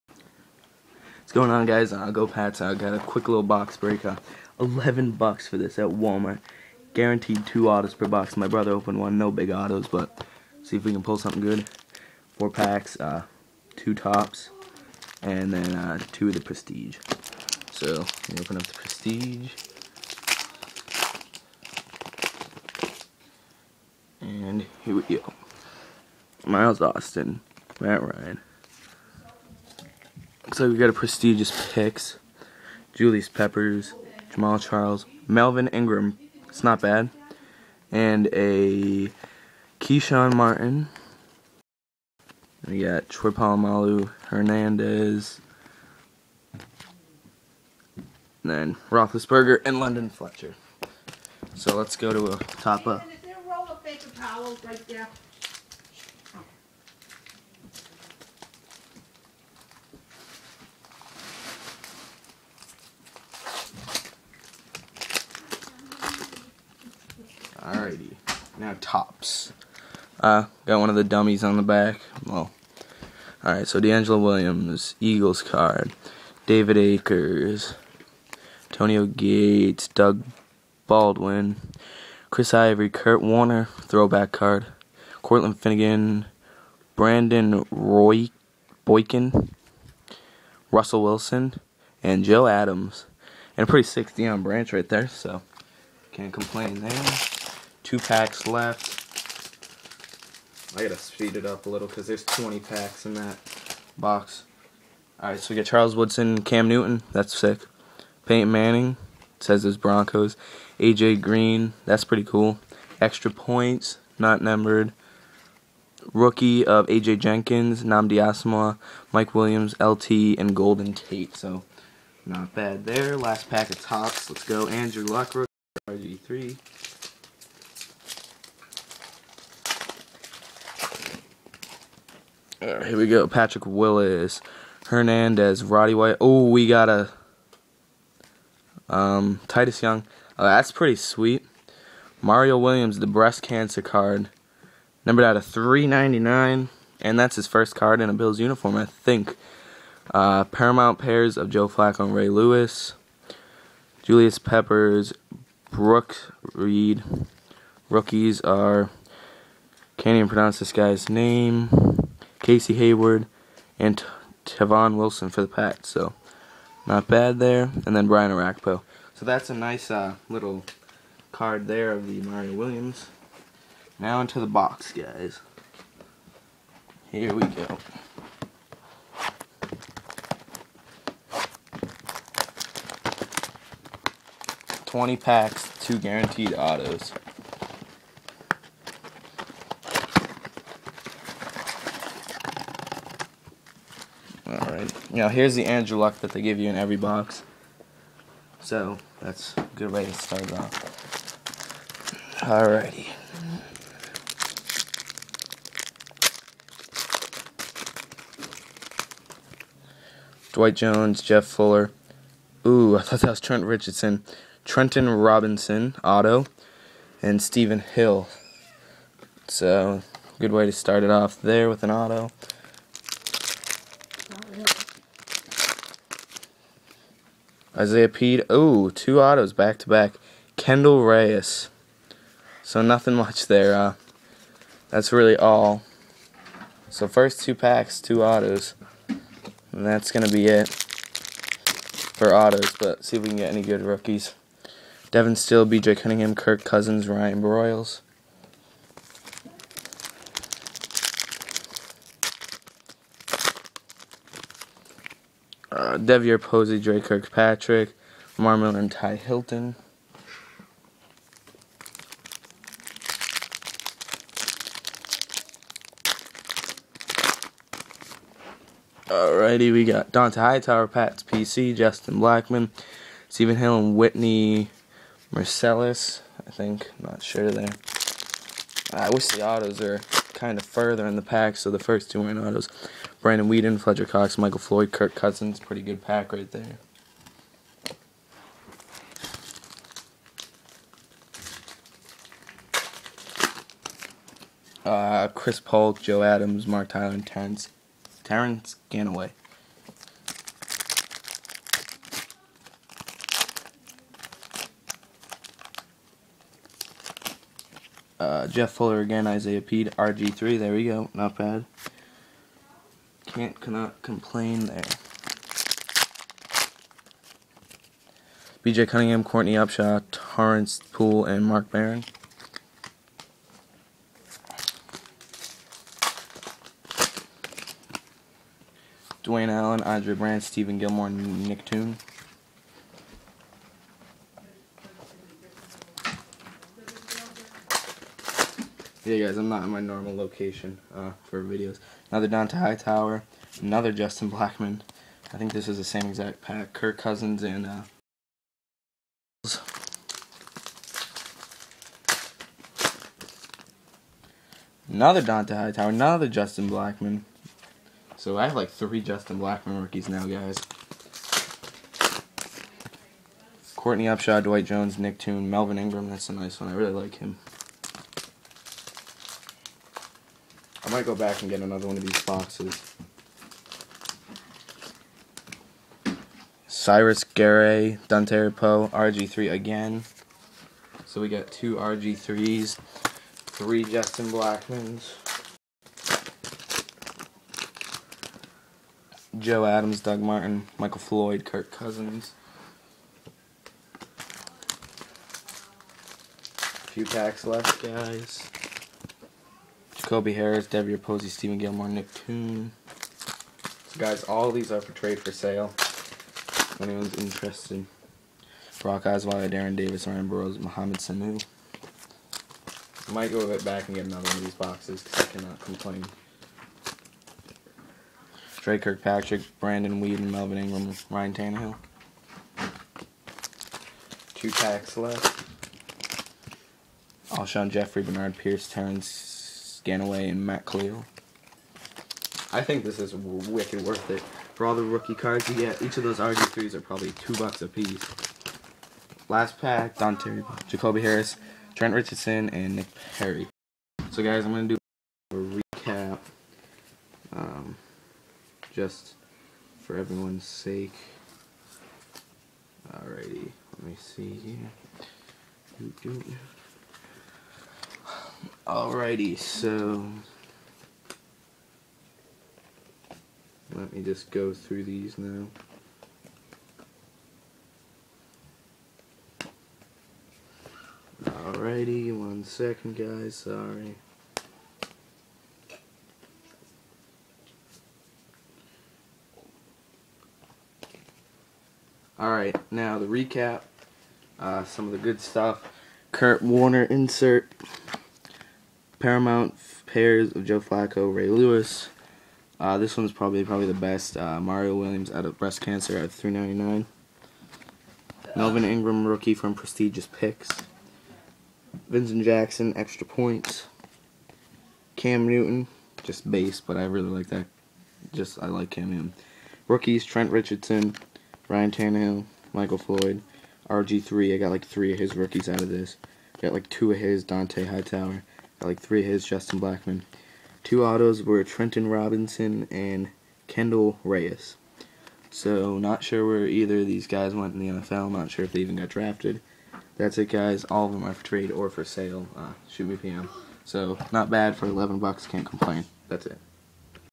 2012 leaf draft and 2 topps and prestige pack break!
Only $11 for the box and got 2 autos! All ft/fs pm me!